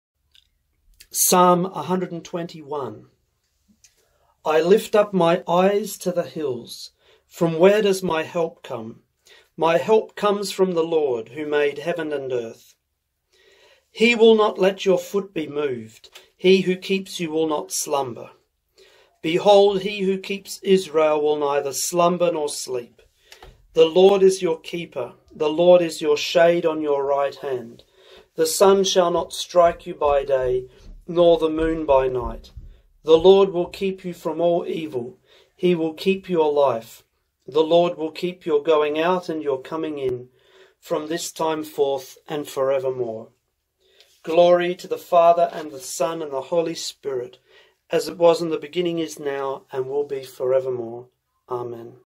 Psalm 121
I lift up my eyes to the hills.
      From where does my help come?
My help comes from the LORD,
      who made heaven and earth.
He will not let your foot be moved;
      he who keeps you will not slumber.
Behold, he who keeps Israel
      will neither slumber nor sleep.
The LORD is your keeper;
      the LORD is your shade on your right hand.
The sun shall not strike you by day,
      nor the moon by night.
The LORD will keep you from all evil;
      he will keep your life.
The LORD will keep
      your going out and your coming in
      from this time forth and forevermore.